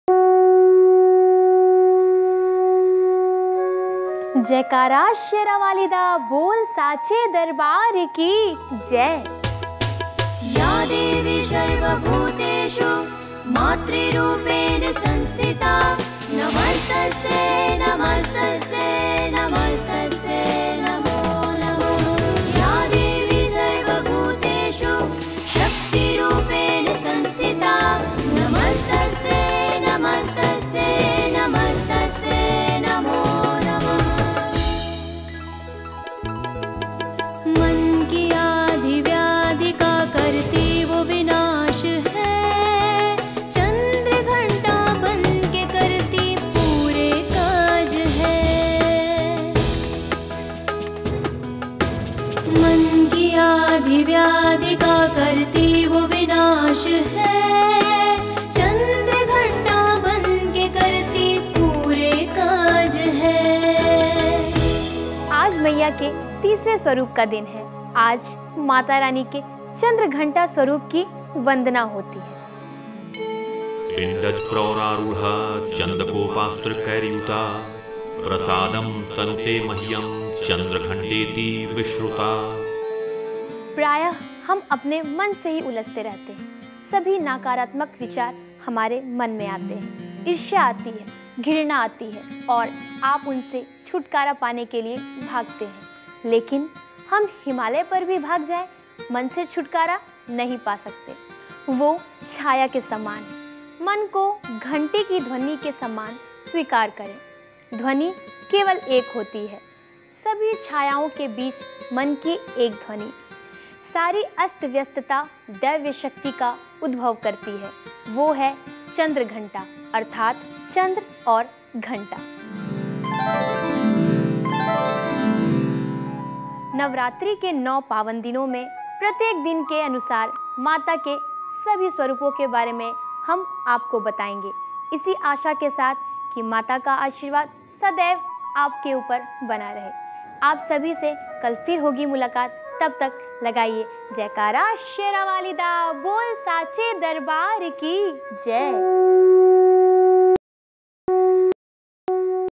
Navratri Day - 3 (Devi Chandraghanta)
Devi Chandraghanta is worshipped on the third day (Tritiya) of Navratri. Her name Chandraghanta means “one who has a half-moon shaped like a bell. Her third eye is always opened and she always ready for war against demons”. The ten-handed Goddess is depicted mounting a tiger. In her four left hands, she is seen carrying Trishula(trident), Gada(mace), khadak (sword) and kamandal (water pot) while the fifth left hand is in Varada Mudra. In her right hands, she holds Kamala (lotus flower), Ghanta (bell), bow and arrow with the fifth right hand in Abhaya Mudra. She is also fond of Jasmine flower and must be offered during puja.